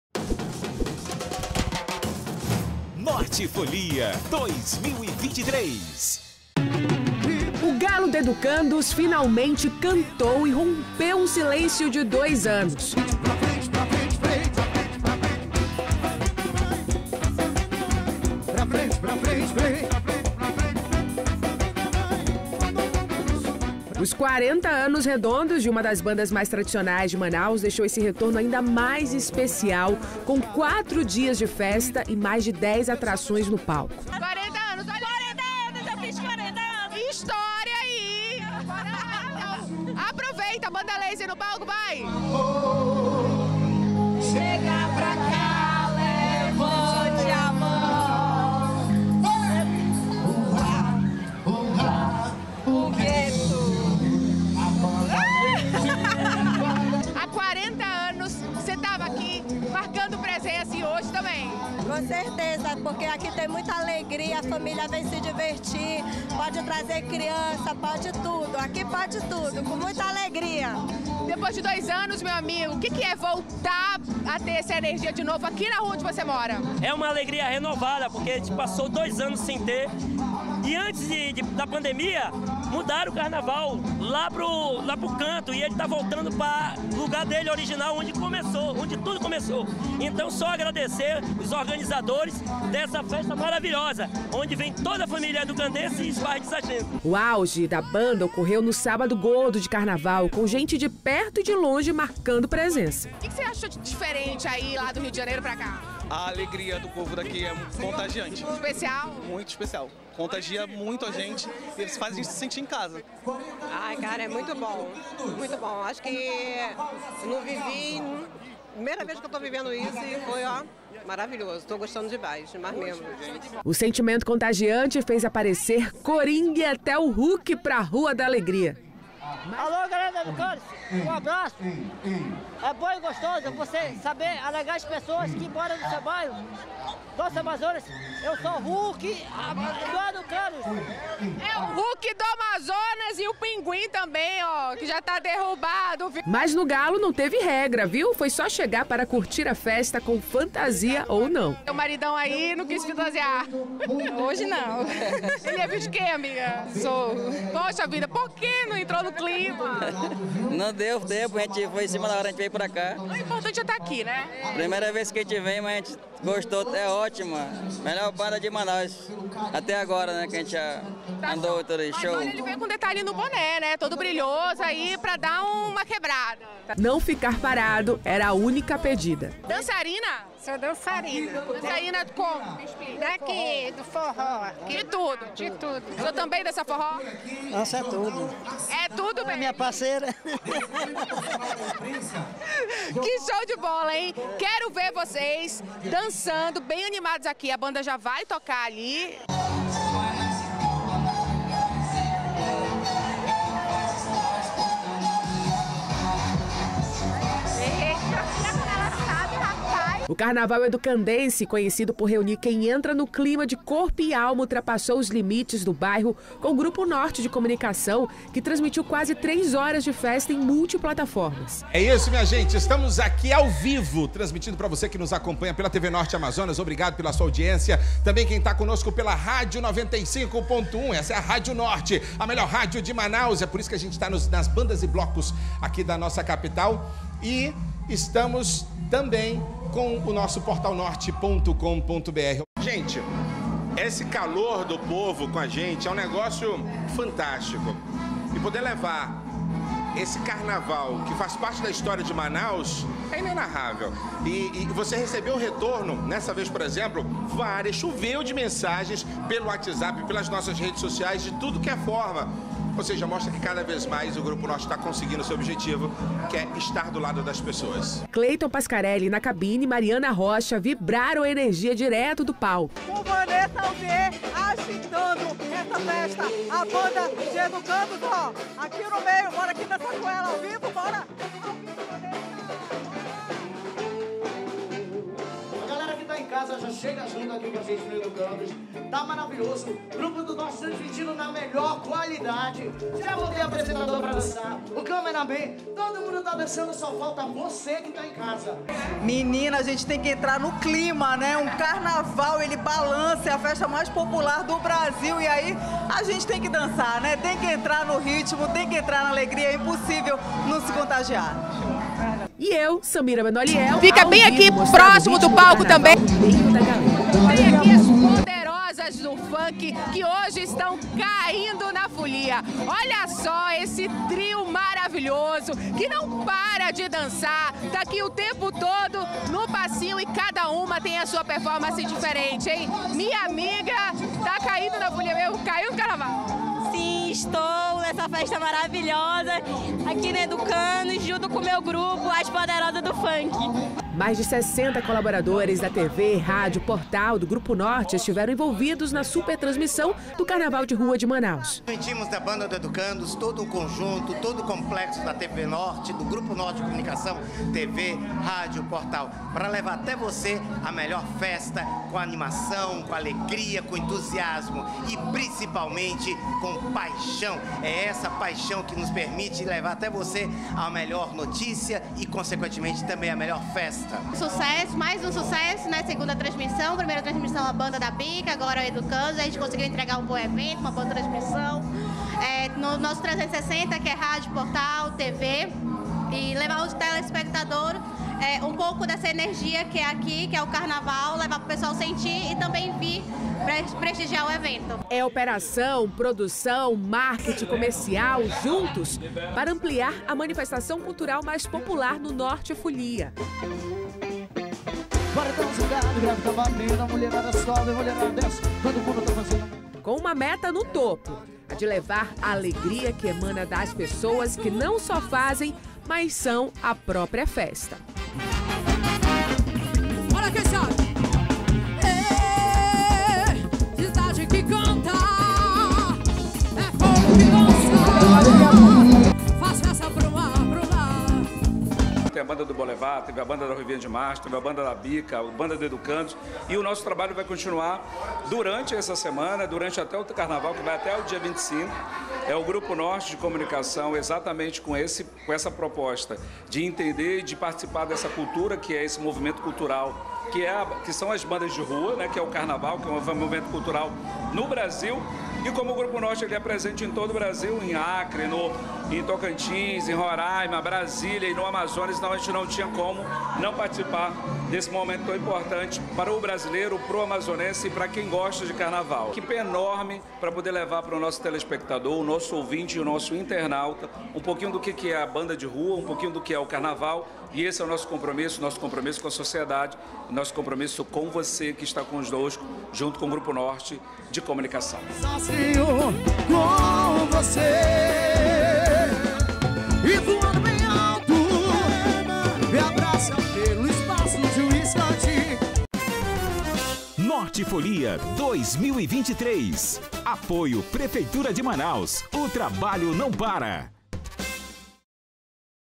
Grupo Norte transmite quase três horas de festa em multiplataformas
🎉O Carnaval do Educandos volta a reunir foliões em Manaus na Orla do Amarelinho depois de um longo período sem festa. E mais um vez, o Grupo Norte de Comunicação marcou presença e transmitiu a festa em multiplataformas: TV, Rádio e Portal.

🎤Reportagem de Samira Benoliel (@samirabenoliel)

Acompanhe!📺